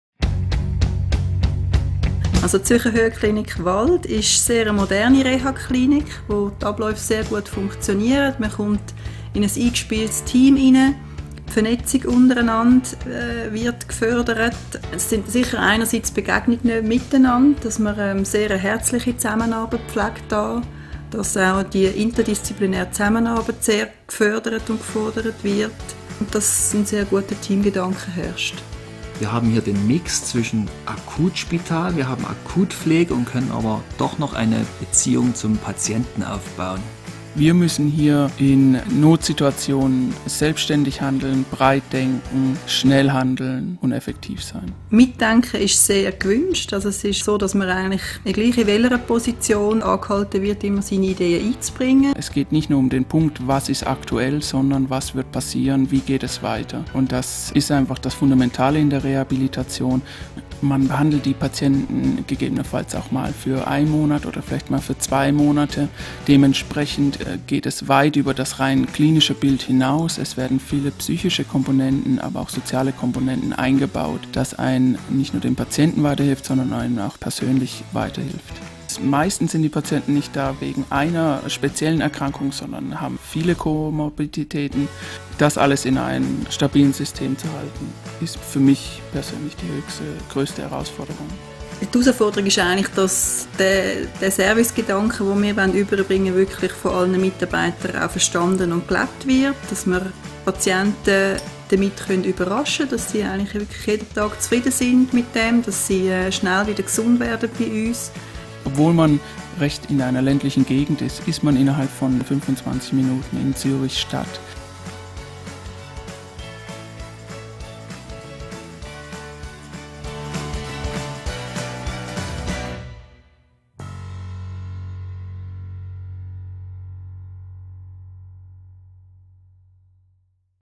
Zürcher RehaZentrum Wald
Bereits vor über 100 Jahren waren Sonne und gute Höhenluft entscheidende Gründe für den Bau der Klinik auf dem Faltigberg (900 m.ü.M.). Besonders am Faltigberg sind die nebelarme Höhenlage, die längste Sonnenscheindauer im Kanton Zürich und der prächtige Panoramablick.